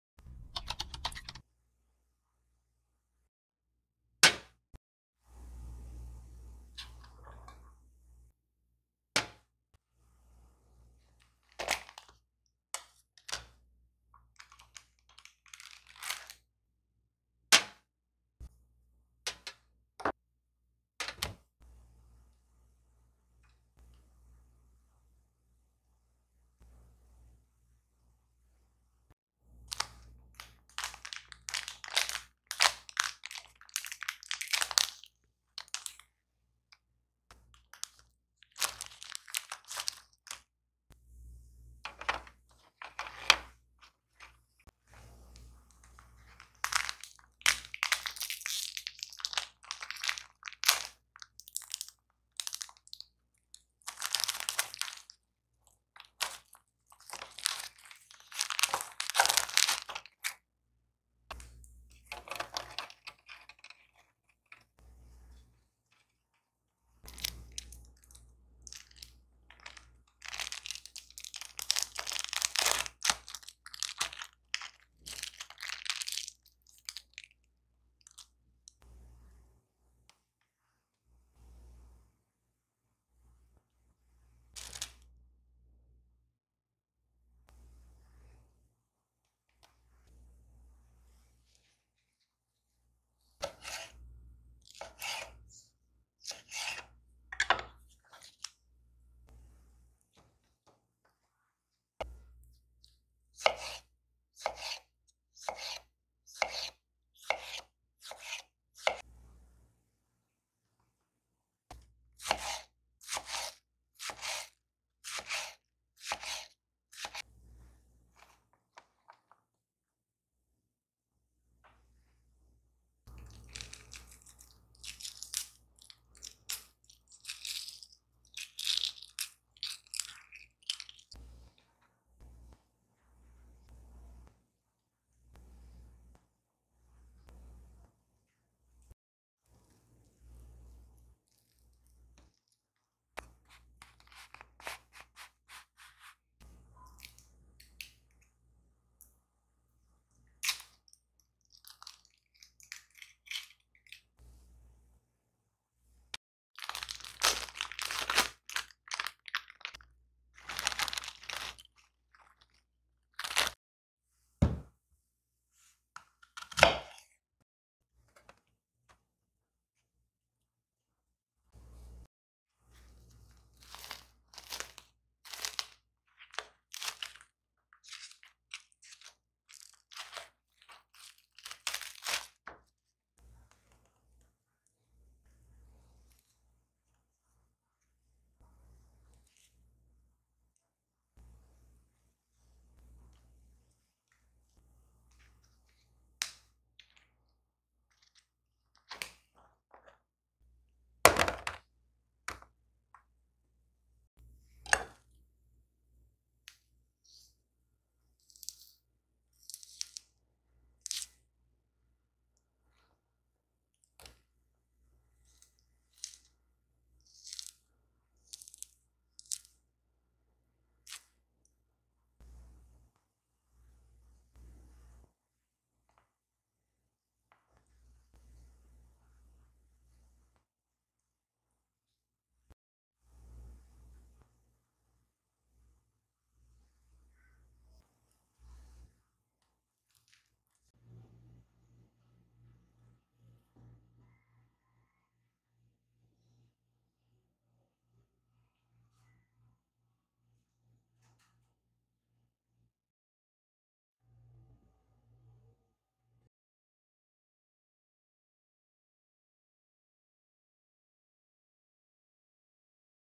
Breakfast Idea for Eid | Charcuterie Board for Eid Breakfast | Kitchen Story
Sharing with you all a unique and trending breakfast idea for Eid. This Eid set up a CHARCUTERIE BOARD for Eid Breakfast or you can also say CHEESE BOARD. Decorate it with Eastern Breakfast items and give your breakfast table a different touch. As much as I enjoyed making video of this board in ASMR style, my kids and I really had fun eating snacks from it. Give it a go and DM your feedback to me on my Instagram :)
Go out of the box, get creative and show your love to your family members and enjoy Eid!! Also, try to avoid large gatherings given the circumstances, stay home stay safe :)
Love from Kitchen Story